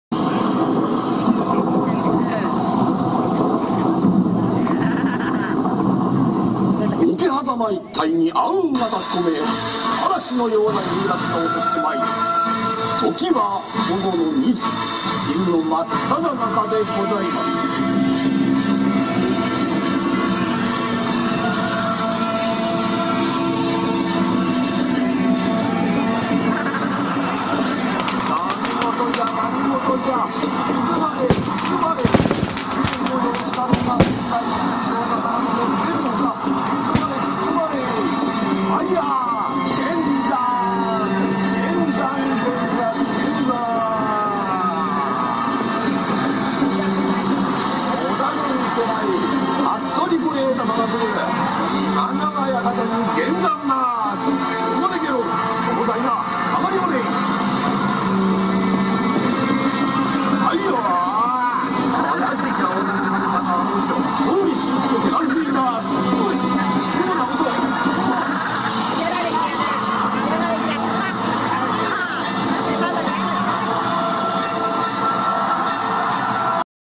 桶狭間古戦場まつり03 今川義元の最期
「刀竜門BASARA」甲冑劇専門サイト

2003年6月8日に行われた「桶狭間古戦場まつり」での義元最期のシーン。毛利新助として登場。